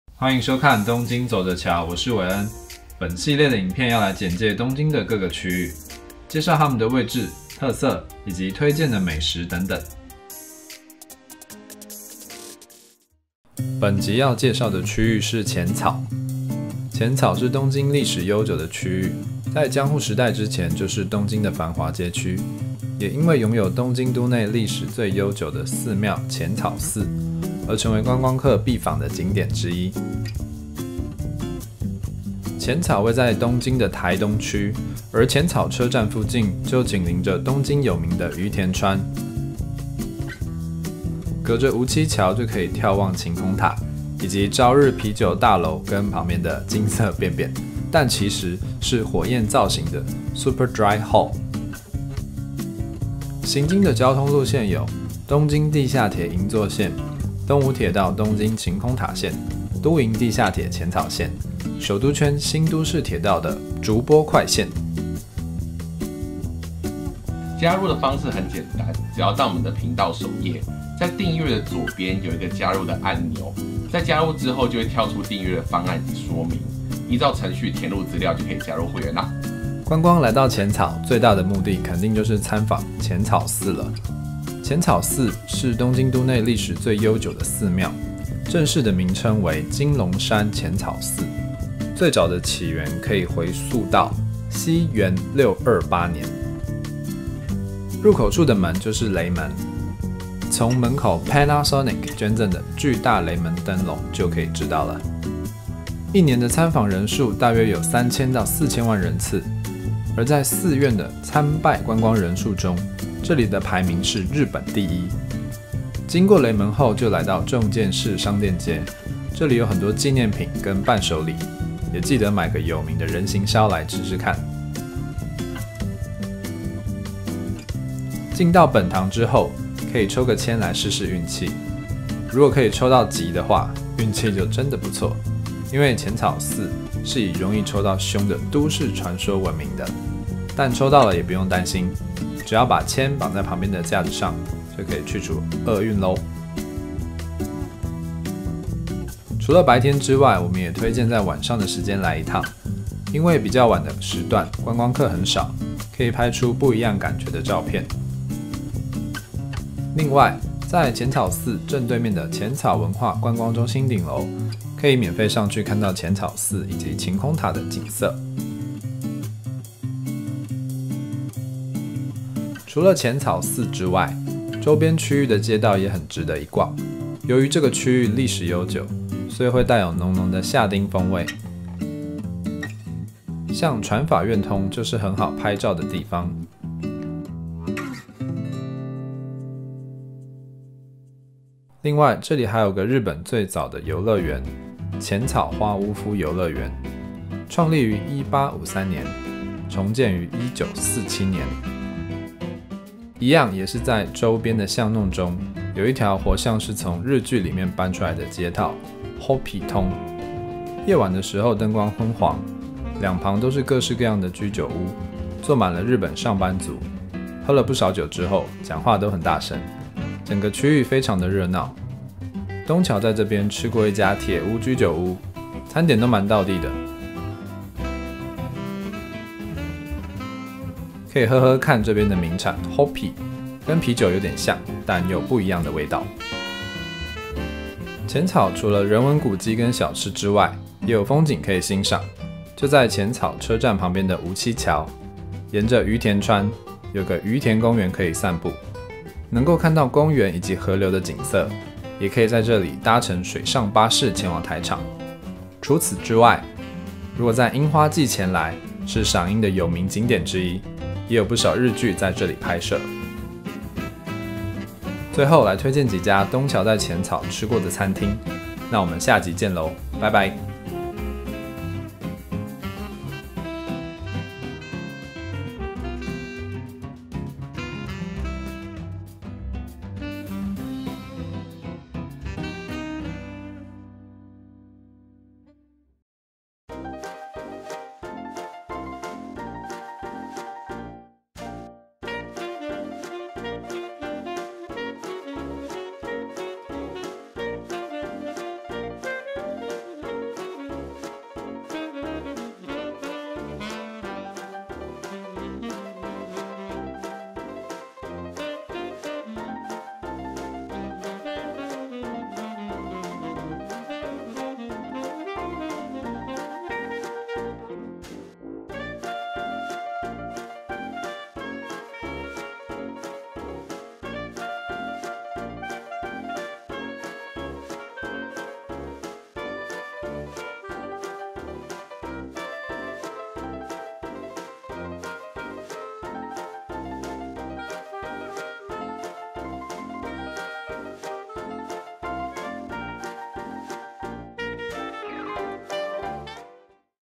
東京區域簡介 - 淺草｜東京自由行
【東京人氣景點｜淺草】
東京區域簡介系列本集要來介紹，東京觀光必訪的聖地「淺草」
除了淺草寺之外還有什麼好玩的呢？